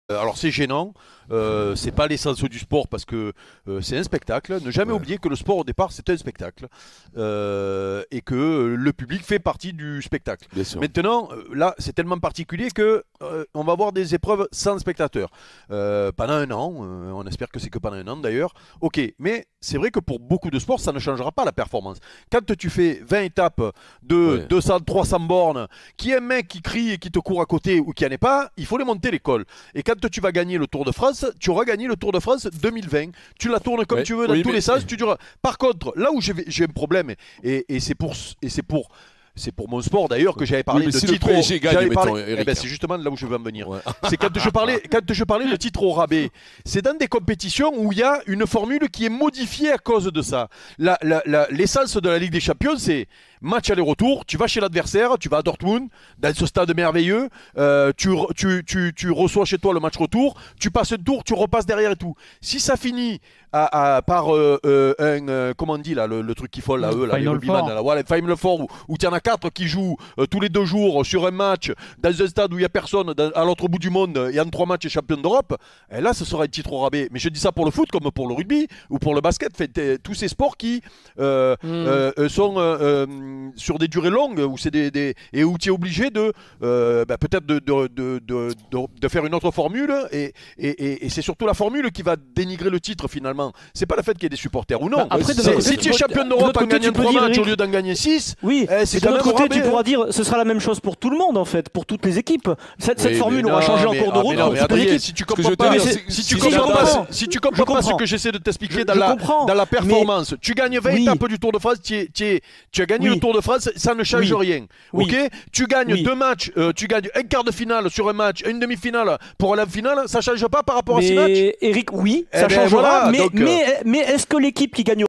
Ligue des champions : Di Meco craint "un titre au rabais" en cas de formule modifiée
Dans les nombreux dossiers à traiter pour l'UEFA à cause de cette crise du coronavirus figure la saison actuelle.